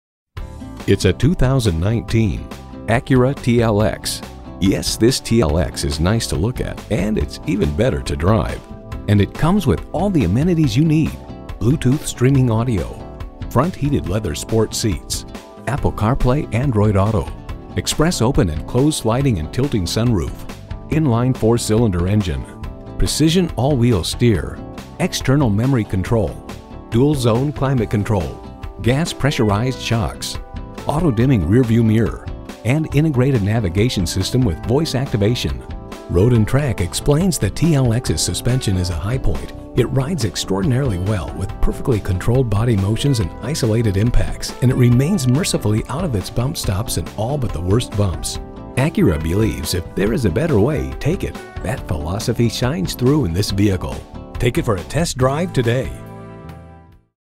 Used 2019 Acura TLX Greenville SC Easley, SC AP5913 - SOLD
Year: 2019
Make: Acura
Model: TLX
Trim: 2.4L Technology Pkg w/A-Spec Pkg
Engine: 2.4L DOHC 16V
Transmission: 8-Speed Dual-Clutch
Mileage: 37173
Stock #: AP5913
VIN: 19UUB1F63KA004376

Address:
2450 Laurens Rd